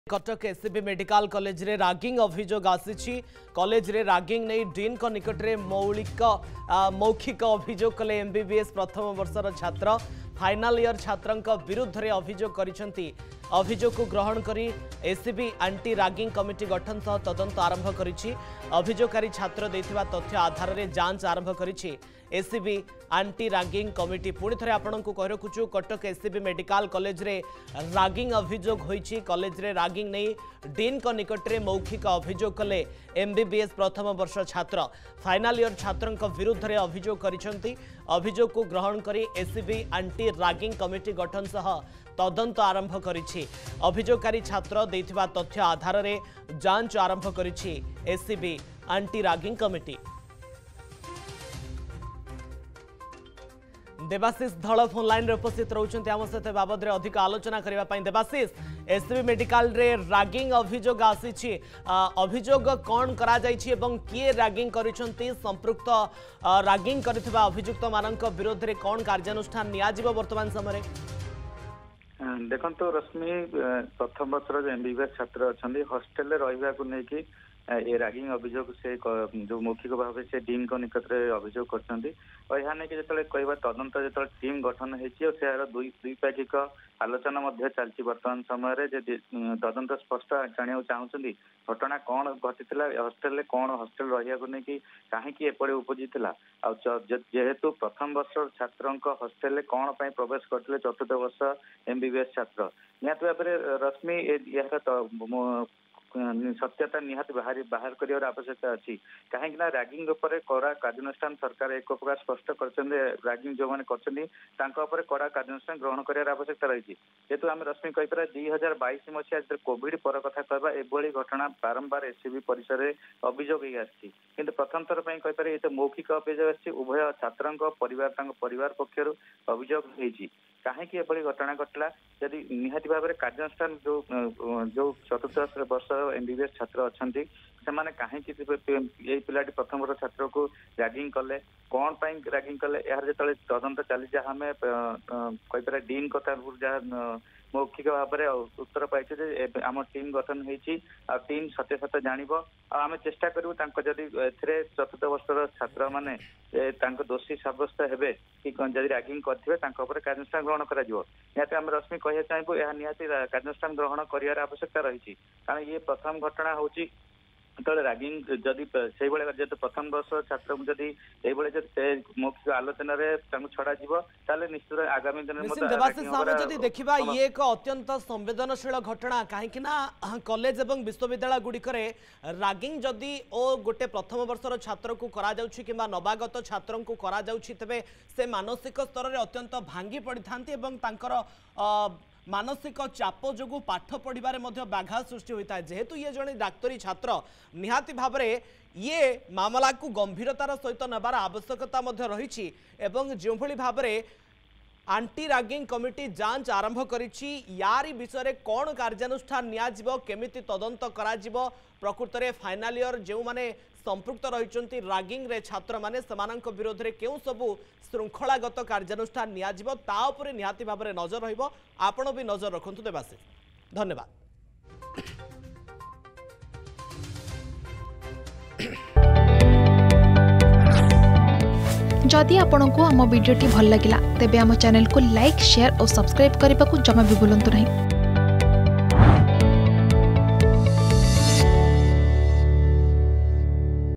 କଟକ SCB ମେଡିକାଲ କଲେଜରେ ରାଗିଂ ଅଭିଯୋଗ । || SCB Medical collage
କଟକ SCB ମେଡିକାଲ କଲେଜରେ ରାଗିଂ ଅଭିଯୋଗ । ରାଗିଂ ନେଇ ଡିନଙ୍କ ନିକଟରେ ମୌଖିକ ଅଭିଯୋଗ । ଫାଇନାଲ ଇୟର ଛାତ୍ରଙ୍କ ନାଁରେ ଅଭିଯୋଗ ଆଣିଛନ୍ତି MBBS ପ୍ରଥମ ବର୍ଷ ଛାତ୍ର ।